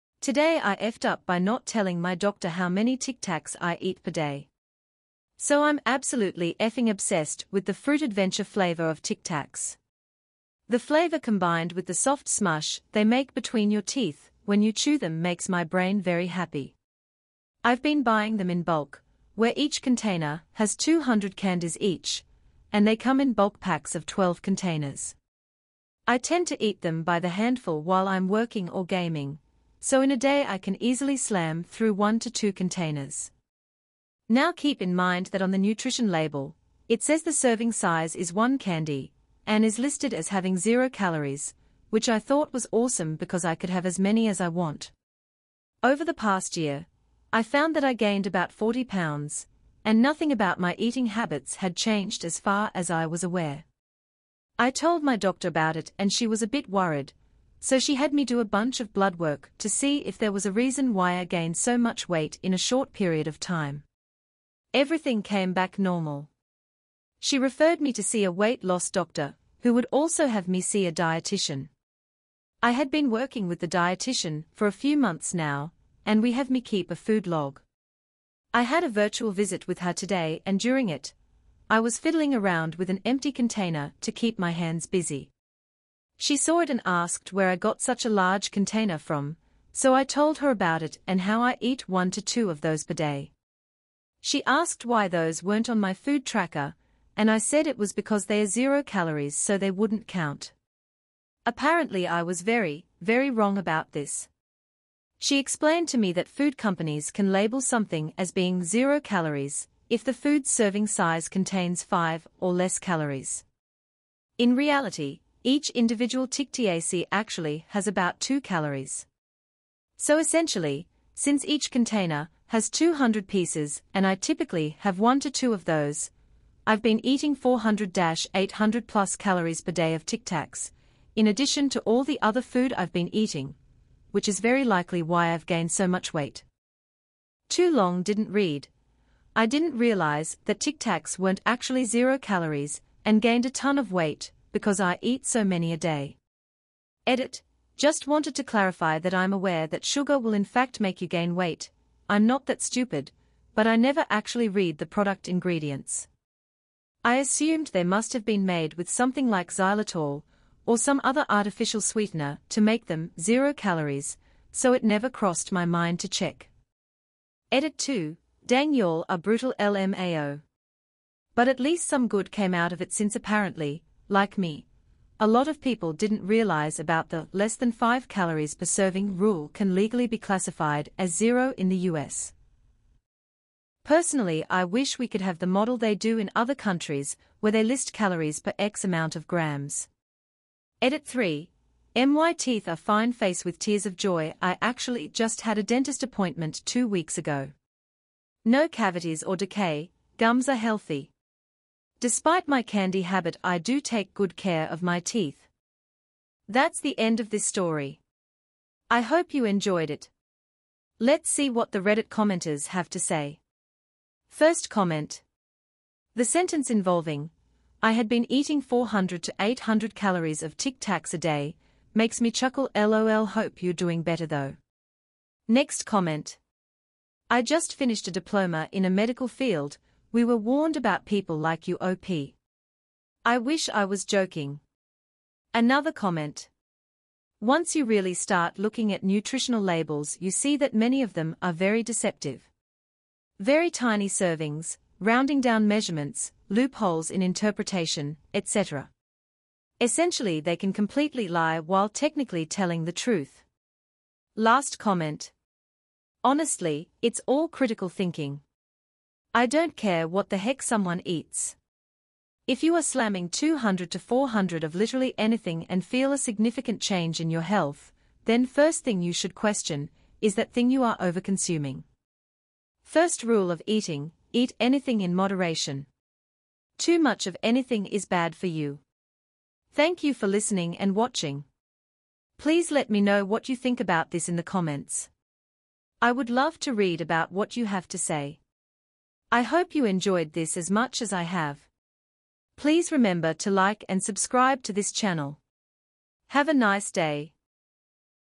Today I Effed Up By Not Telling My Doctor How Many Tic-Tacs I Eat Per Day
r/tifu
u/DumpsterPuff

I love to make videos to make you laugh.
Stay healthy and cheerful!